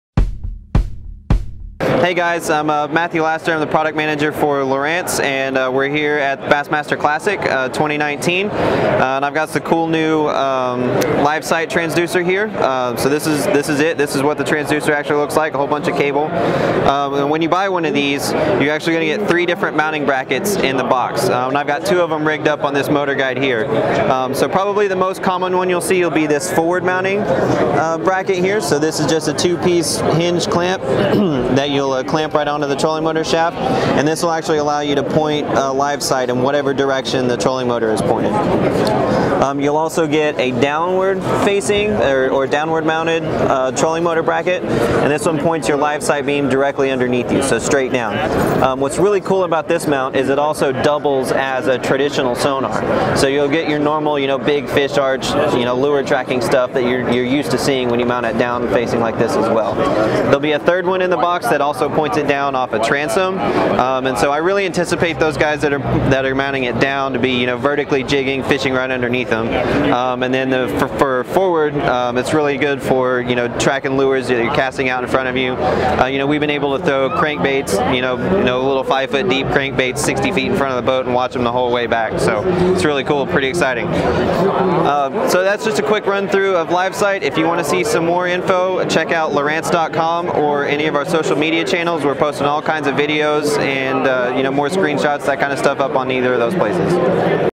Matthew Laster with Lowrance on the new LiveSight Transducer with mounts at the Bassmaster Classic
Matthew Laster with Lowrance on the new LiveSight Transducer with mounts at the Bassmaster Classic. Sonar Fishing Fanatic. Video by Brad Wiegmann Media.